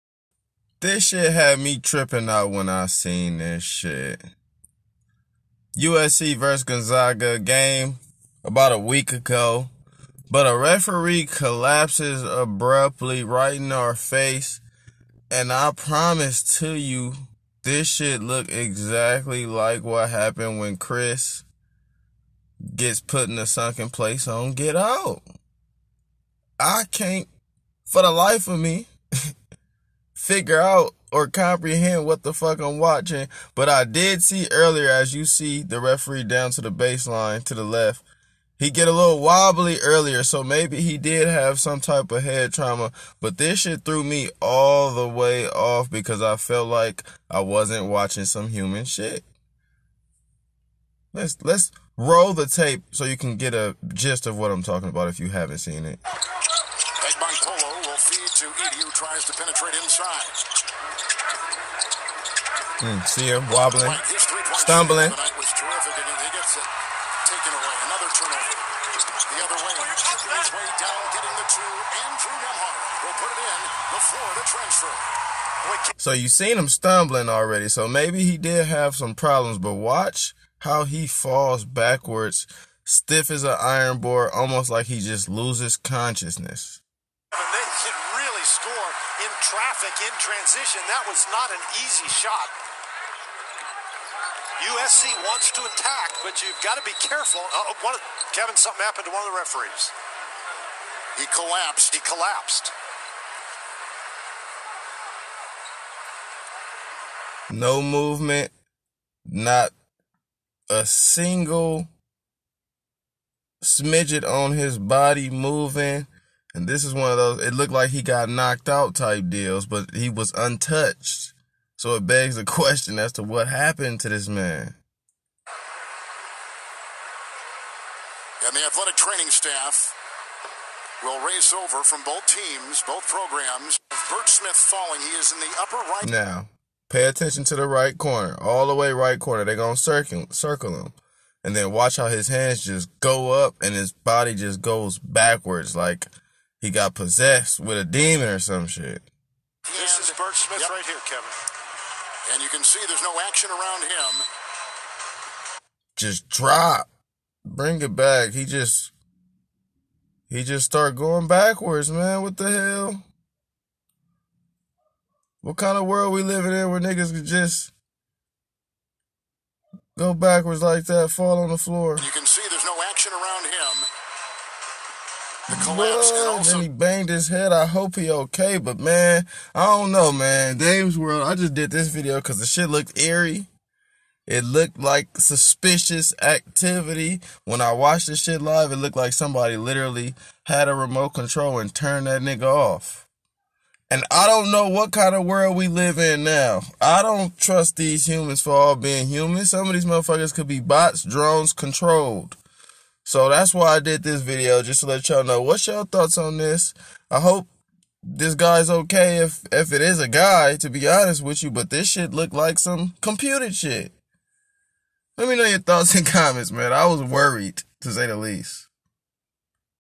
Referee Bert Smith Collapses on the Court!! Dame responds!! Suspicious Activity??
Dame gives a brief breakdown of referee falling on his ass. Thoughts welcomed